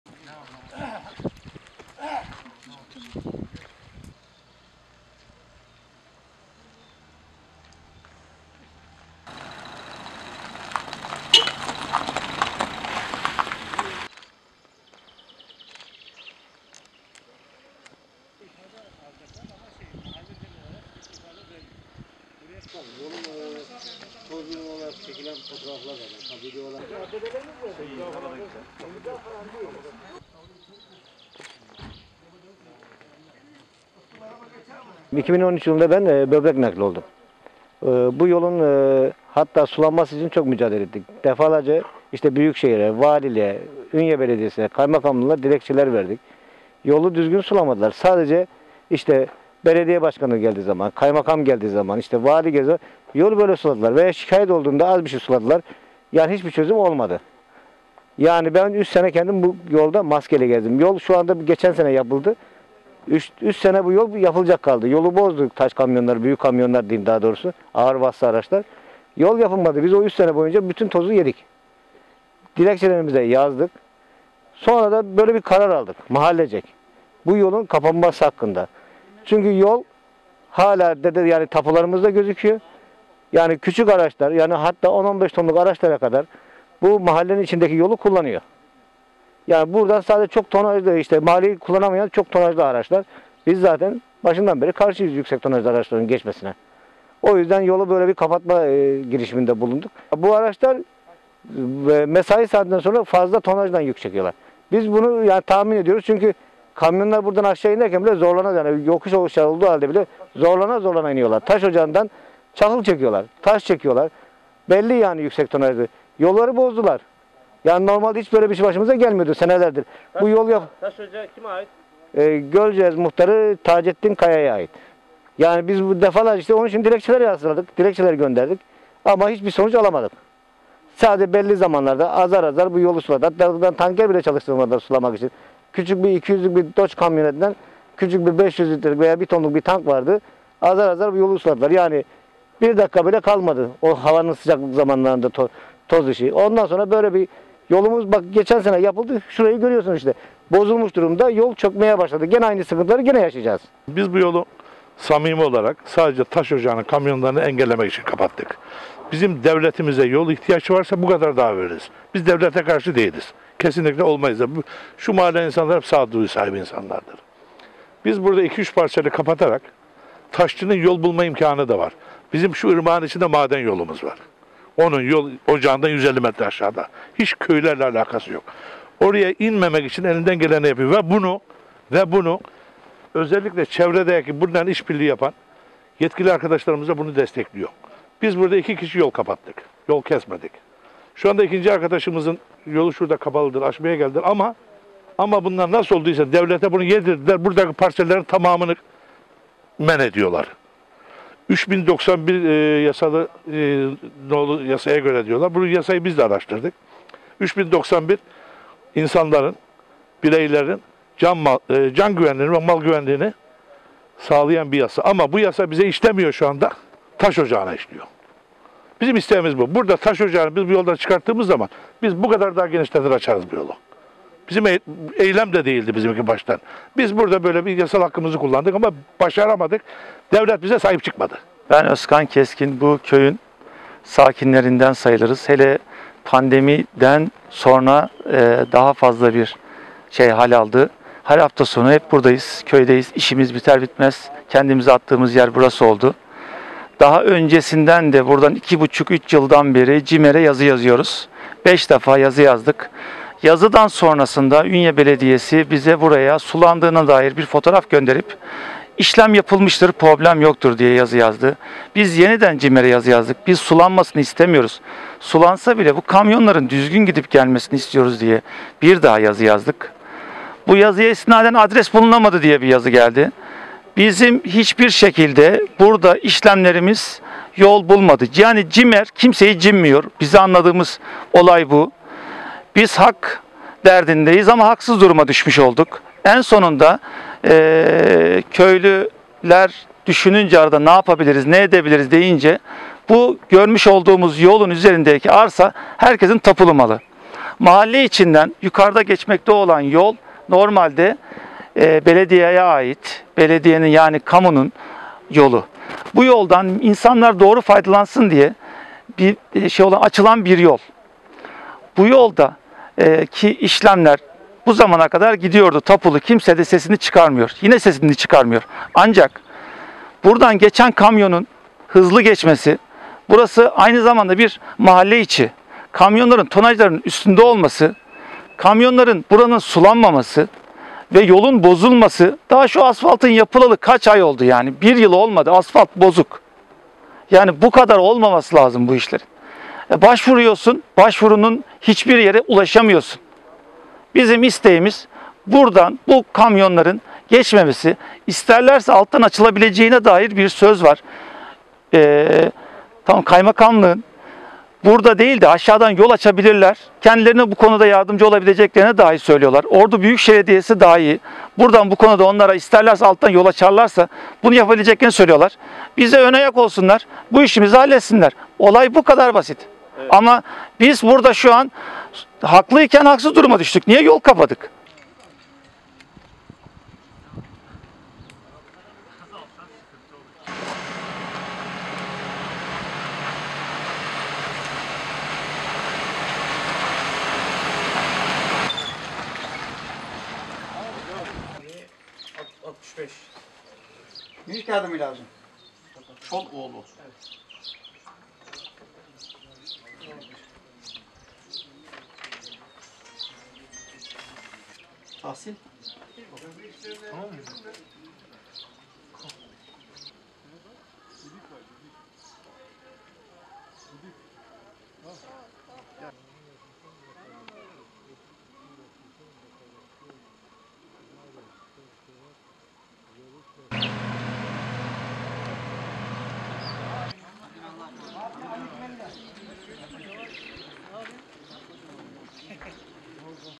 Devlet Bize Sahip Çıkmadı
Ünye'nin Keş Mahallesi Hatipli Sokak üzerinde yaşayan vatandaşlar, kendi tapu yollarının olmasına rağmen yüksek tonajla geçen araçların geçişine izin vermeyerek sorunun bir an önce çözülmesini istedi.

Keş Mahallesi Hatipli Sokak'ta yaşayan vatandaşlar, yıllardır kendilerine ait ve tapulu olduğu öğrenilen asfalt yoldan yüksek tonajlı kamyonların geçmesinin ardından mağduriyet yaşadıklarını söylediler. Özellikle bir şahsa ait olduğu öğrenilen ve mesai saatleri sonrası yüksek tonajlı kamyonların geçmesiyle asfalt yollarının bozulduğunu, meyve ağaçlarının kuruduğunu iddia eden mahalle sakinleri sorunun bir an önce çözülmesini istedi. Yaz ayının gelmesiyle beraber evlerinde duramadıklarını söyleyen mahalle sakinleri, devletin yetkililerine seslenerek yeni bir yolun yapılması konusunda çağrıda bulundu.

"Yüzlerce Dilekçe Yazdık Bir Sonuç Alamadık"

Mahalle içerisinden yüksek tonajlı kamyonların geçmesinden dolayı yaşadıkları sıkıntıları anlatan mahalle sakinlerinden Hasan Kılıç," 2013 yılında ben böbrek nakli oldum. Biz bu yolun sulanması için çok mücadele ettik. Ordu Valiliği'ne, Kaymakamlığa ve Ünye Belediyesi'ne dilekçeler verdik. Yolu düzgün sulamadılar. Sadece belediye başkanı, kaymakamlar ve valiler geldiği zaman sulama yaptılar. tabi şu zamana kadar kalıcı olarak hiç bir çözüm bulunamadı. Ben üç sene bu yolda maskeli gezdim. Bu yol şu anda geçen sene yapıldı. Dilekçelerimizi de yazdık. Şu an üzerinde bulunduğumuz bu yol tapularımızda bize ait olarak görünüyor. Küçük olan araçlar yani 10-15 tonluk araçlar bu arkamızdaki ufak yolu kullanıyor. Onun dışında yüksek tonajlı araçların geçmesine karşıyız. O yüzden bu yolu kapatma girişiminde bulunduk. Bu araç dediklerimiz mesai saati sonrası yüksek tonajla yoldan geçmeye çalışıyor. Ve kamyonlar buradan inerken bile zorlanarak iniyorlar. Taş ocağından çakıl ve taş çekiyorlar. Zaten kamyonların geçmesinden tonajları belli oluyor. Bu bahsettiğimiz taş ocağı Gölceğiz Muhtarı Tacettin Kaya'ya aittir. Biz bu durumla alakalı yüzlerce dilekçe yazdık ancak bir sonuç alamadık. Geçen sene bu yol yapıldı, bu gün ise böyle bozuk duruma döndü" dedi.

Yaşadıkları mahallede devletin sahip çıkmadığını söyleyerek yeniden yetkililere çağrıda bulunan bir başka mahalle sakini Muhsin Aydın," Biz bu yolu taş ocağına giden kamyonları engellemek için kapattık. Bizim devletimize yok ihtiyaç varsa bir bu kadar daha veririz. Biz devletimize karşı değiliz. Şu mahallemizin insanları hep sağ duyuludur. Biz burada 2-3 parseli kapatırsak taş ocağının kendisine açacağı yolu da var. Bizim şu karşı mahallede maden yolumuz var. Onun taş ocağı da hemen 150 metre aşağısında kadar duruyor.  Oraya inmemek için elinden geleni yapıyor. Biz burada iki kişi yol kapattık ve yol kesmedik. Biz burada bir eylem yapmak istedik ancak başaramadık. Çünkü devlet bize sahip çıkmadı" diye konuştu.

"CİMER Kimseyi Cimmiyor"

Defalarca yaşanılanları tek tek CİMER'e anlattıklarını ve gelen cevapların yetersiz olduğunu söyleyen bir başka mahalle sakini Özkan Keskin ise," Biz her hafta sonu bu mahallemiz dinlenmeye geliyoruz. Daha öncesinde yaklaşık 2,5- 3 yıldan beri CİMER'e yazı yazıyoruz. Beş defa yazı yazdık. Ancak yazıdan sonrasında Ünye Belediyesi buranın sulandığına dair bizlere fotoğraf gönderip işlem yapıldığına dair yazı yazdı. Biz yeniden CİMER'e yazı yazdık. Çünkü buranın sulanmasını değil, kamyonların düzgün tonajlı gidip gelmesini istiyoruz. Yine CİMER'e bildirdik. Bu seferde adres bulunamadı diye cevapla karşılaştık. Bizim hiç bir şekilde burada işlemlerimiz çözüme kavuşmadı. Yani CİMER kimseyi çimmiyor. Bu durumdan anladığımız olay bundan ibarettir. Biz hak derdindeyiz ama haksız duruma düşmüş olduk. En sonunda köylüler ne yapabilir dediklerinde tapulu olan yolumuzu kapatma kararı aldık. Çünkü burası bir arsa. Yukarda mahalle içinden geçmekte olan yol normalde belediyeye ait. Bu yoldan insanlar doğru faydalansın diye açılan bir yol oldu. Tabi yine kimse sesini çıkarmıyor. Ancak buradan geçen kamyonun hızlı geçmesi mahalle içi olduğu için yolun bozulmasıyla beraber hepimizi mağdur etti. Bizim isteğimiz bu kamyonların buradan geçmemesidir" şeklinde konuştu.